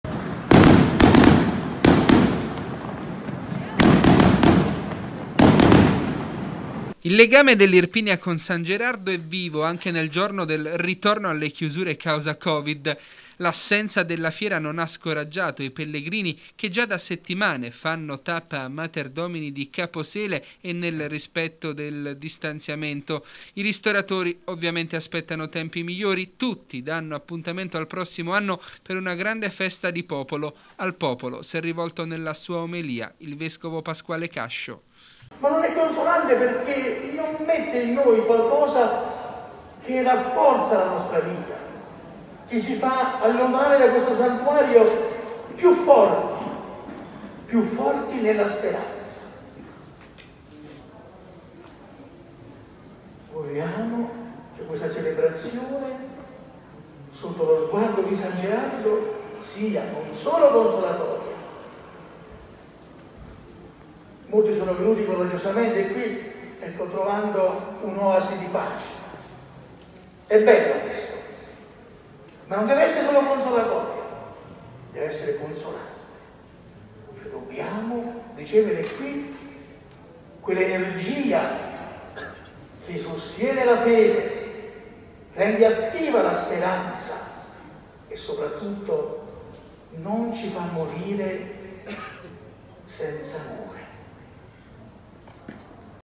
La benedizione da San Gerardo nel giorno delle restrizioni, il vescovo: «energia e sostegno»
Niente Fiera a Materdomini ma non mancano i fedeli nel rispetto dei distanziamenti. Il Vescovo Cascio dall’altare invoca energia e sostegno per fedeli e famiglie. L’appuntamento al prossimo anno per una grande festa